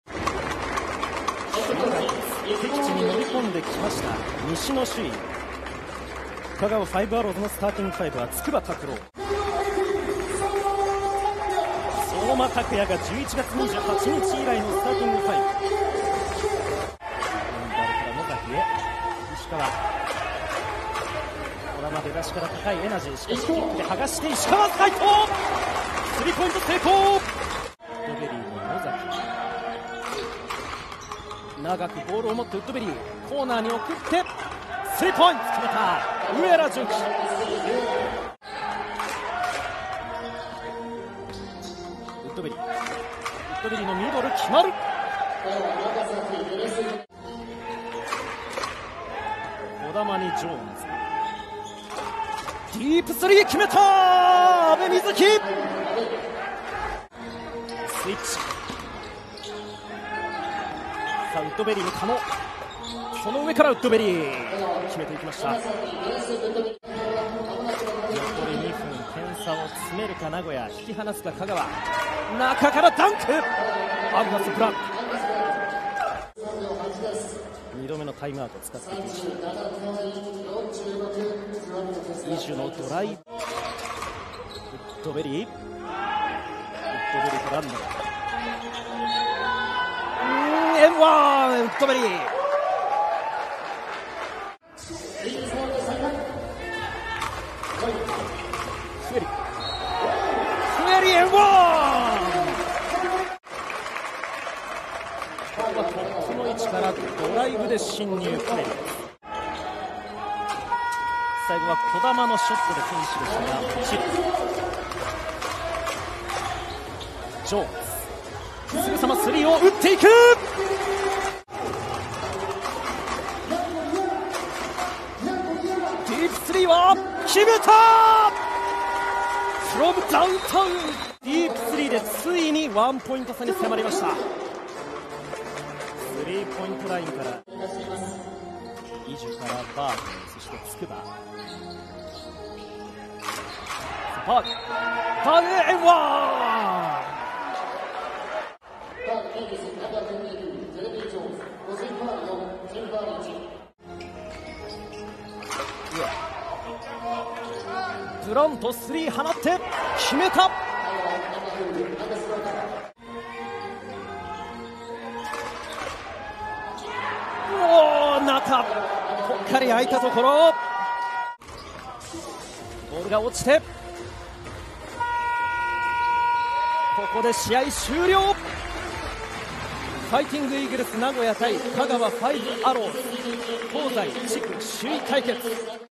【ハイライト】ファイティングイーグルス名古屋vs香川ファイブアローズ｜B2第22節GAME1｜02.19.2022 プロバスケ (Bリーグ)
Watch the Game Highlights from Fighting Eagles Nagoya vs. Kagawa Five Arrows, 02/19/2022